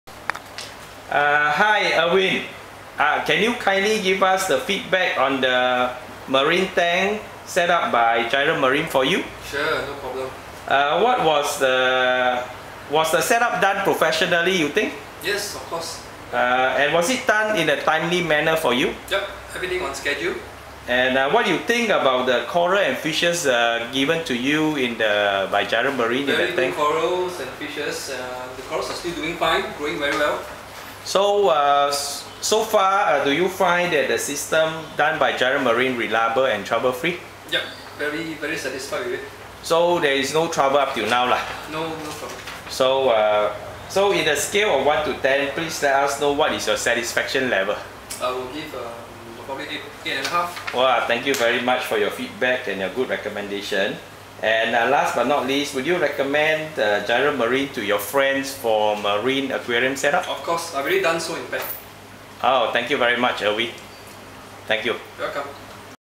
Tank Revamp: Dr. Teo's 8 ft Tank (Full Review)
Dr Teo was delighted with his newly revamped tank after engaging Jireh Marine's service. View his full feedback.